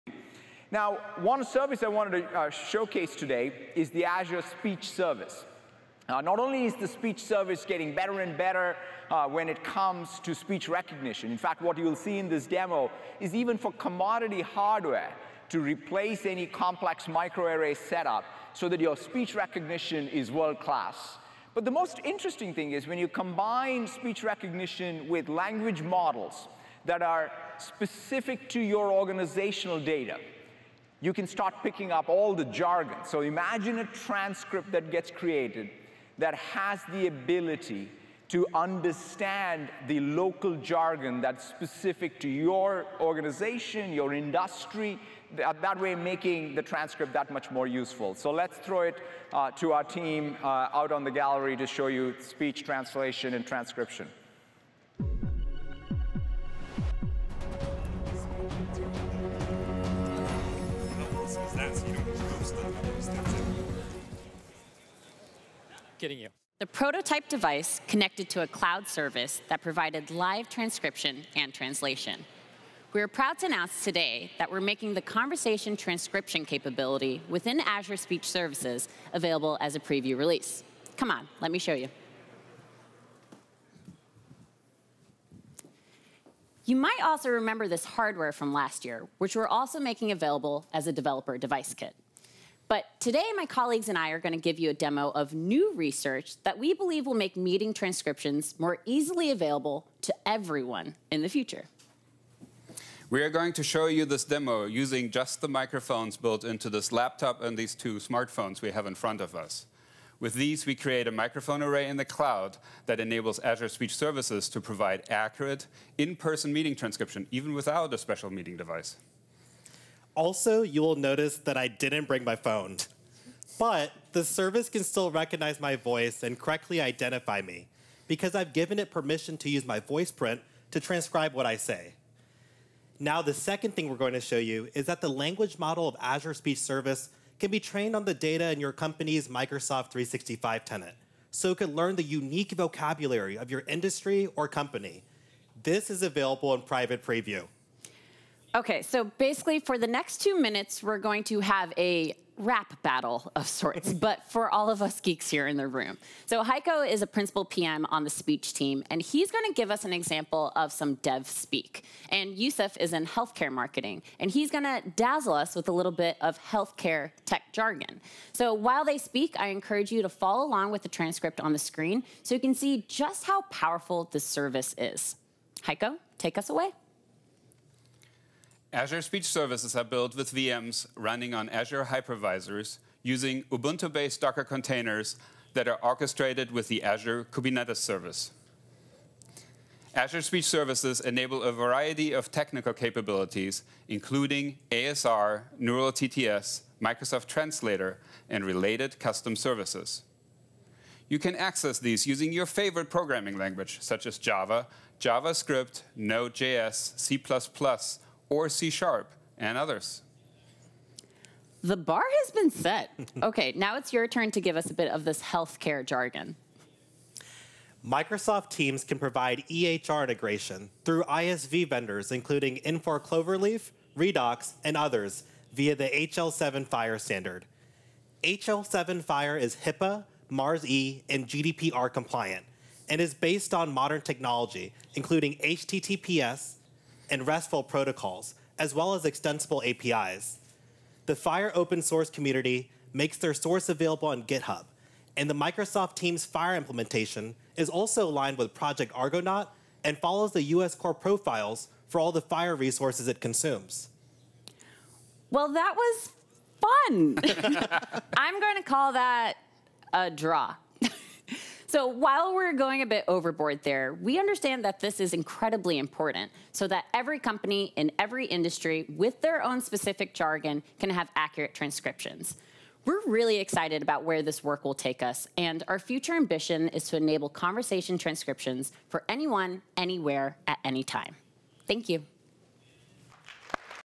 Azure Speech Service Vision Keynote Demo // Microsoft Build 2019
Our latest research progress enables dynamic creation of a virtual microphone array with a set of existing devices equipped with ordinary microphones. It helps our customers more easily transcribe conversations at anytime anywhere. Also, Azure speech service can automatically generate custom speech models using Office 365 data to deliver more accurate transcription on organization-specific vernacular such as technical terms and people names.